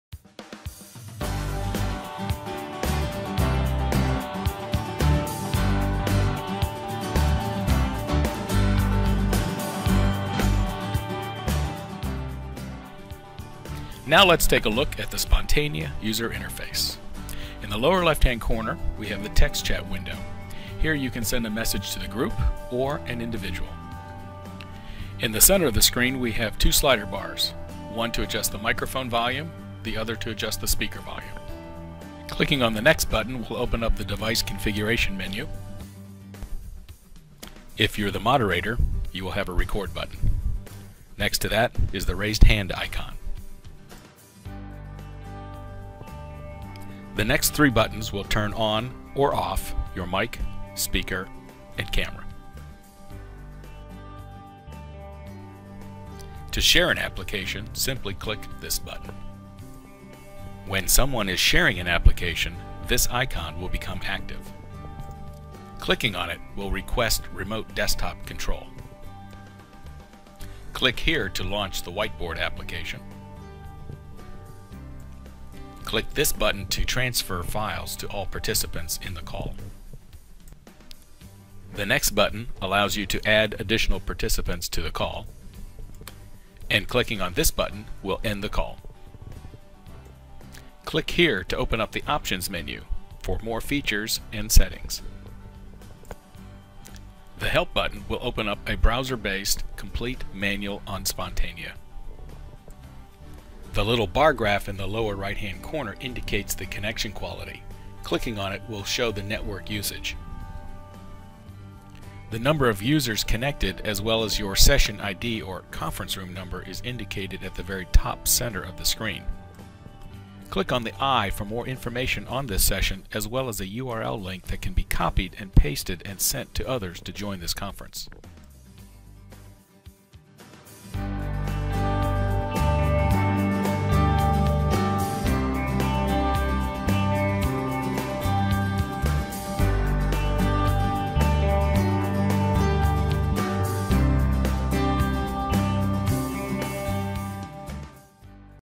Spontania Training #2 – Interface Overview (Jan.2015)
Spontania is a group video conferencing software, with exceptional video and
voice quality that includes a suite of real time collaboration tools so that you can
share and meet on-line in an effective and productive way with anyone, anywhere.